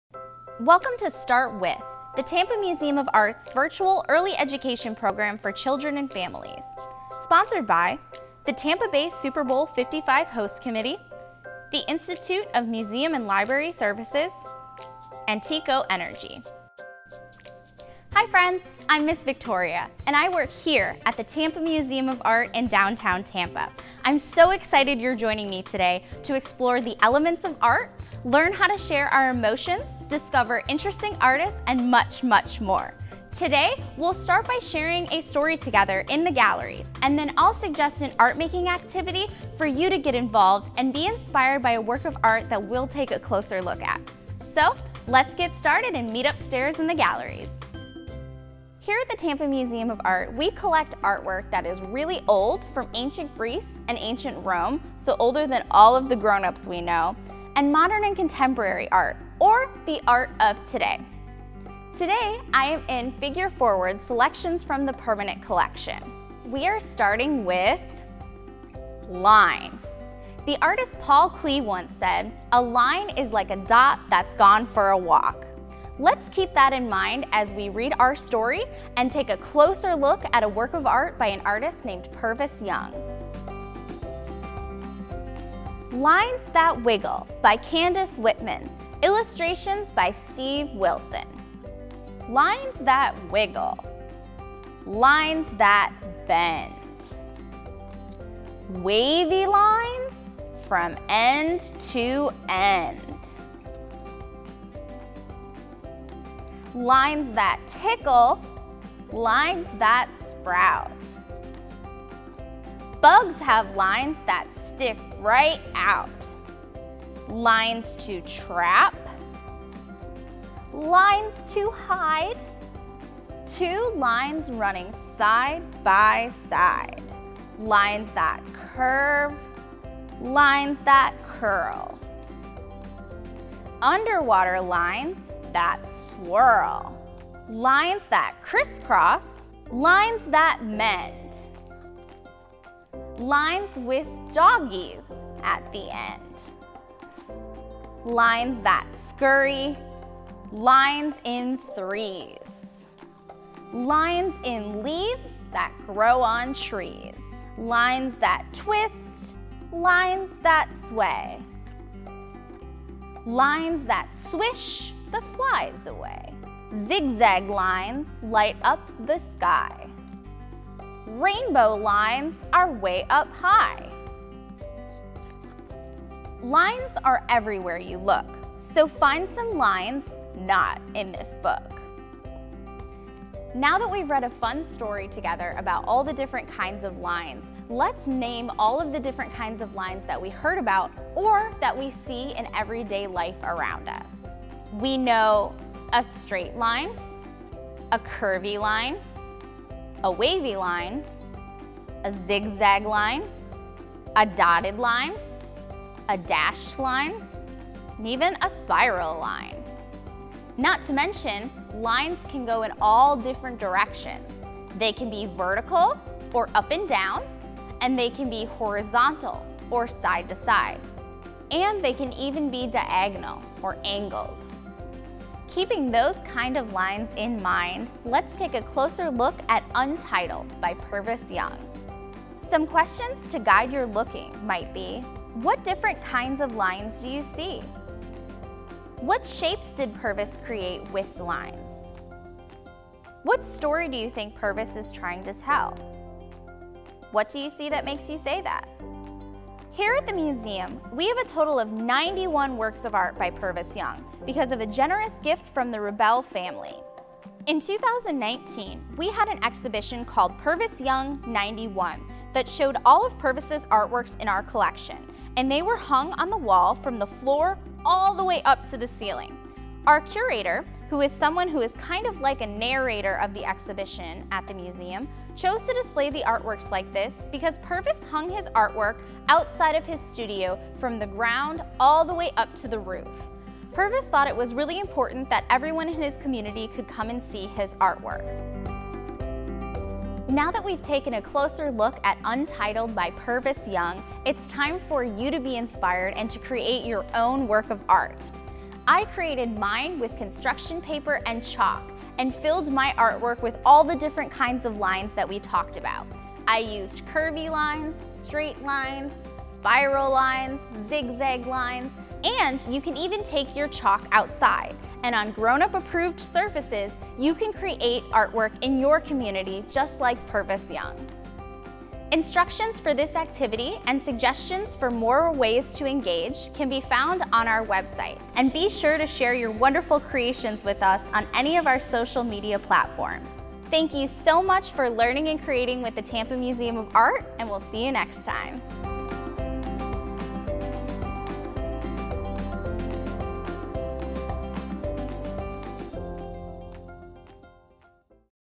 stART With... Lines! Purvis Young & "Lines That Wiggle"
Each StART With episode features a new subject with virtual story time and an art activity designed by the Tampa Museum of Art education team. Children will create and explore with the elements of art, learn how to share emotions, discover interesting artists, and much more!

In this episode, we read "Lines That Wiggle" by Candace Whitman with Illustrations by Steve Wilson. View more information and follow along with supplemental materials at TampaMuseum.org/Learn/Children-Families.